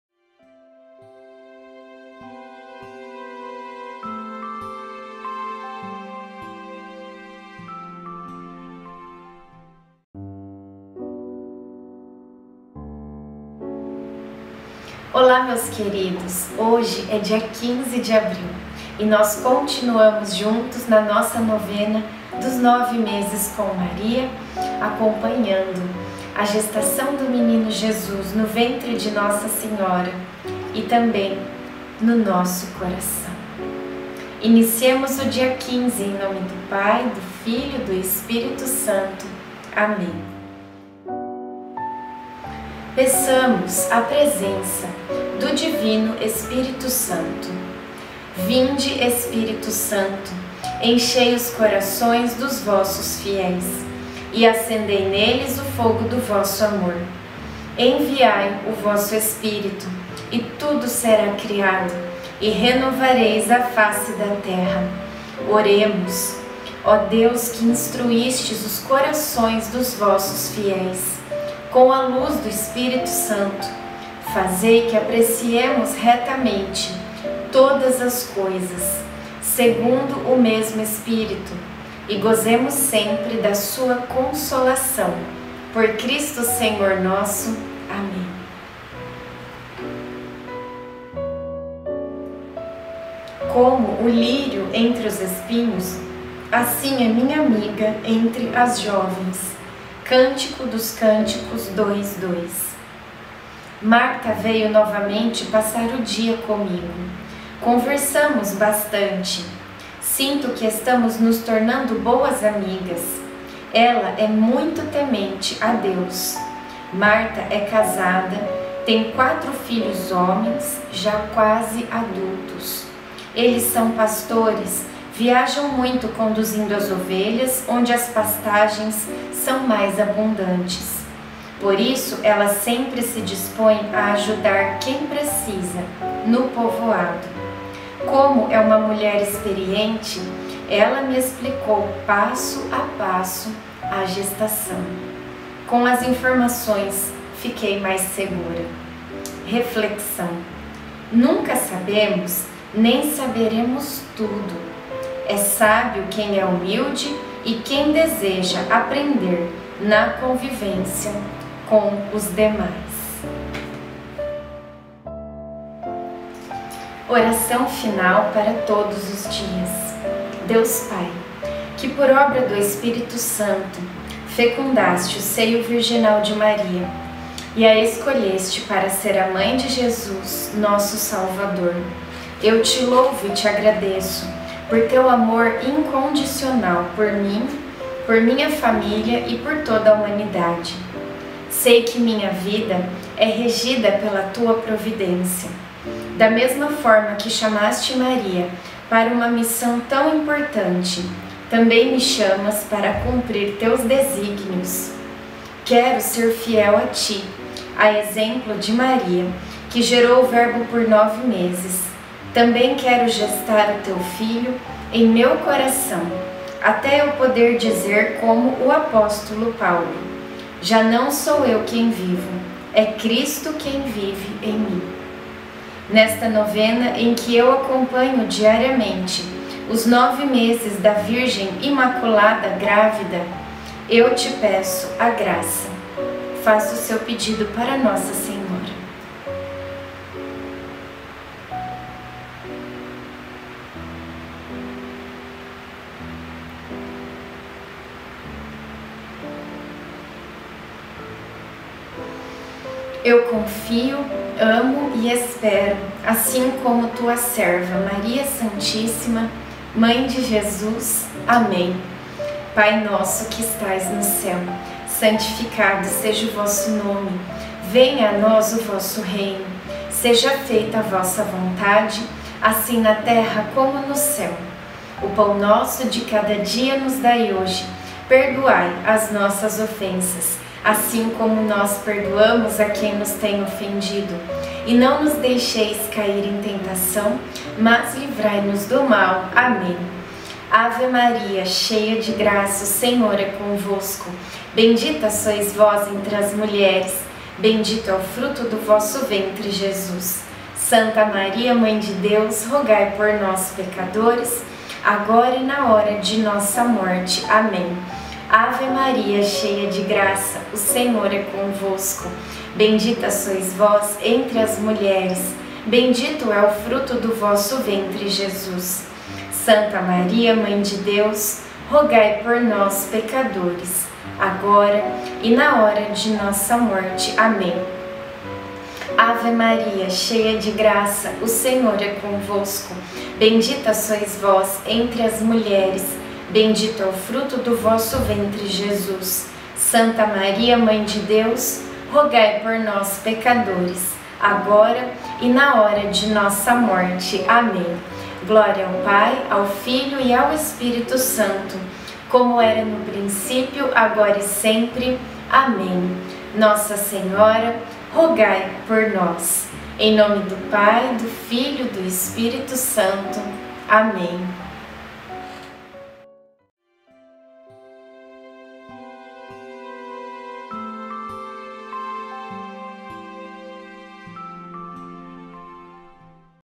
NOVENA 9 MESES COM MARIA - 15/04
15 de abril

Como o lírio entre os espinhos, assim é minha amiga entre as jovens. (Cântico dos

Cânticos 2,2)

Marta veio novamente passar o dia comigo, conversamos bastante. Sinto que
estamos nos tornando boas amigas. Ela é muito temente a Deus. Marta é casada,
tem quatro filhos homens, já quase adultos. Eles são pastores, viajam muito
conduzindo as ovelhas onde as pastagens são mais abundantes. Por isso ela sempre
se dispõe a ajudar quem precisa no povoado. Como é uma mulher experiente, ela
me explicou passo a passo a gestação. Com as informações fiquei mais segura.
Nunca sabemos, nem saberemos tudo, é sábio quem é humilde e quem deseja

aprender na convivência com os demais.

doações espontâneas

Deus abençoe!
Link: (388) Patrícia Acosta Cozinha Comigo - YouTube